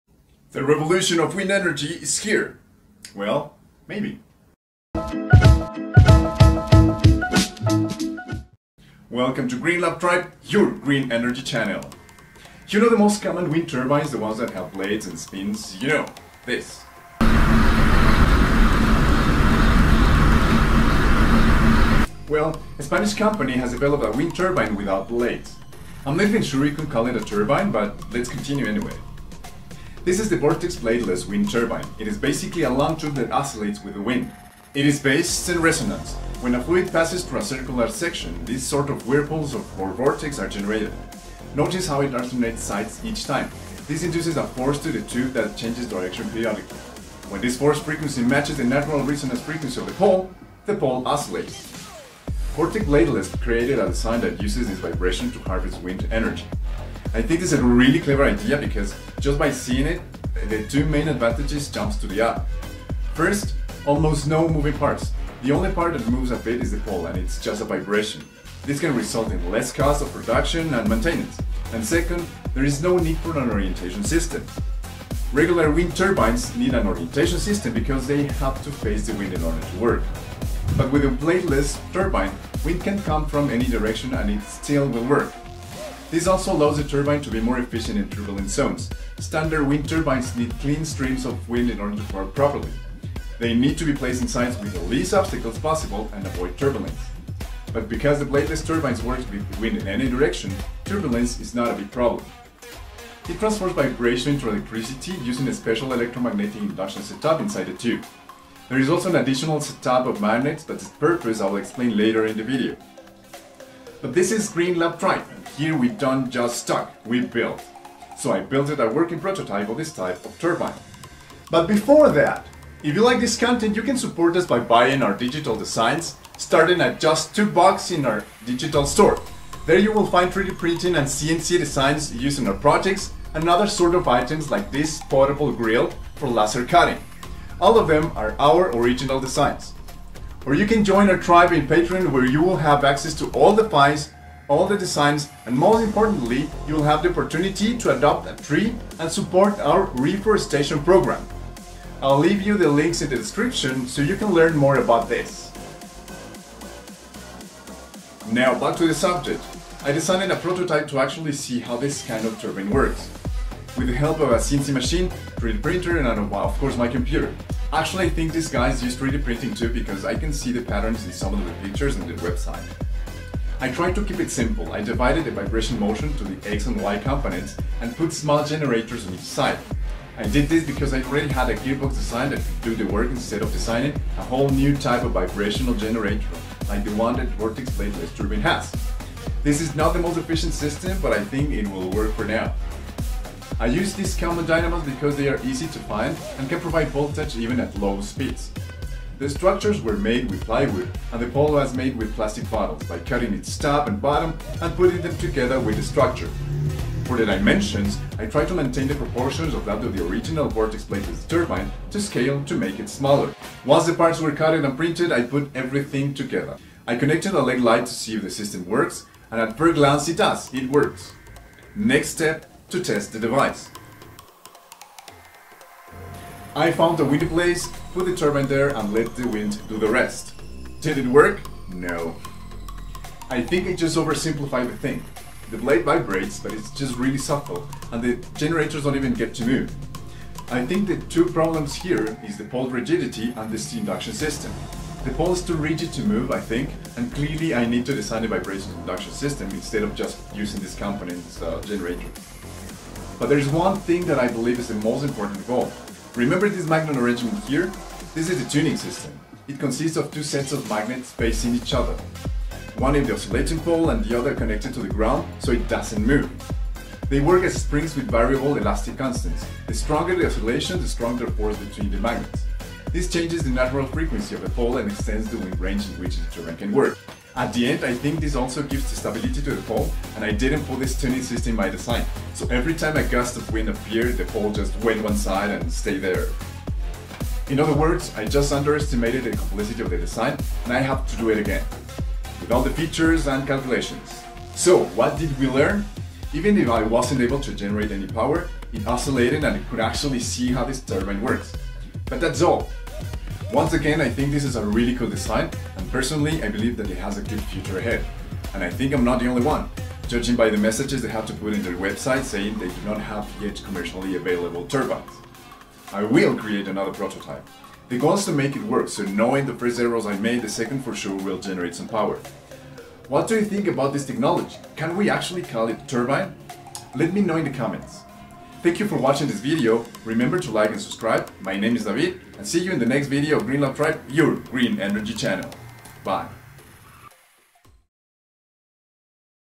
How to harvest energy from wind? without blades!
The Vortex bladeless turbine is a bladeless wind turbine that combines resonance and vortex generation in the air to harvest  energy from wind. If you ask yourself how to innovate in wind energy, this can be the answer.  It can be one of the weirdest wind turbines and here  in Green Lab Tribe we took a first try to build a prototype of this type to actually see how it works. It didn’t go as planned but we learned some things. The first one, even when it looks really simple, the design of this kind of bladeless wind turbine is not. I believe that the Vortex bladeless company is on the good path to launch some interesting technology.